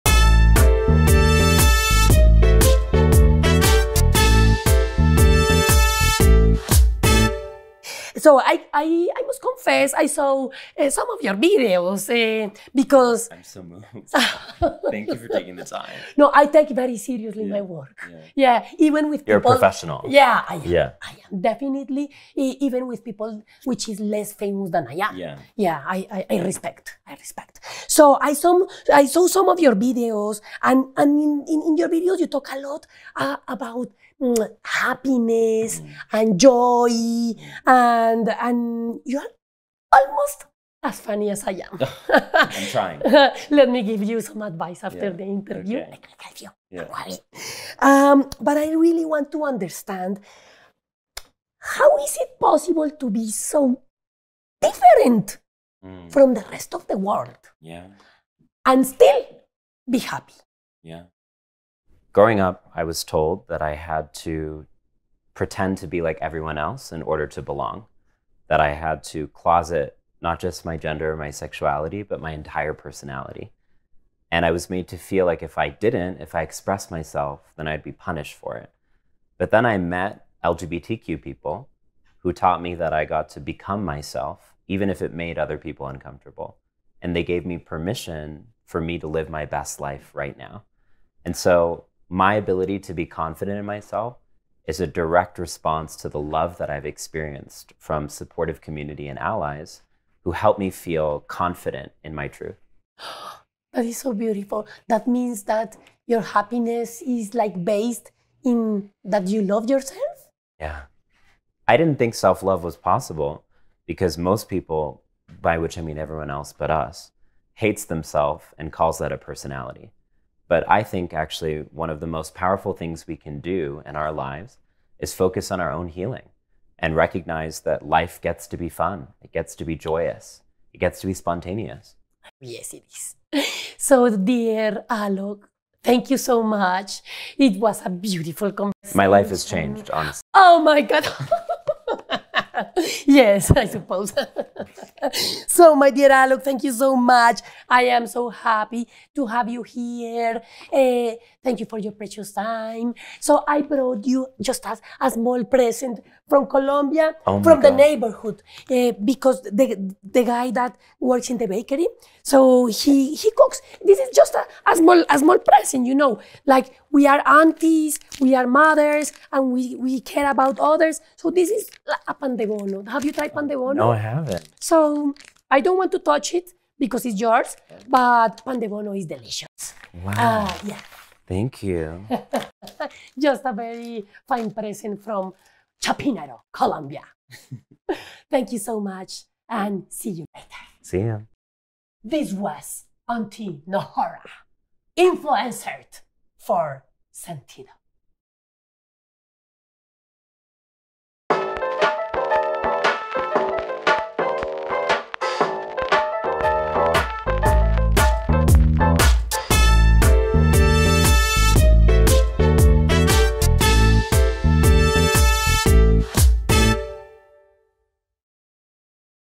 Así cierra la Tía Nohora su entrevista con Alok
Así cierra la Tía Nohora su entrevista con Alok Menon por encargo de las de Sentiido.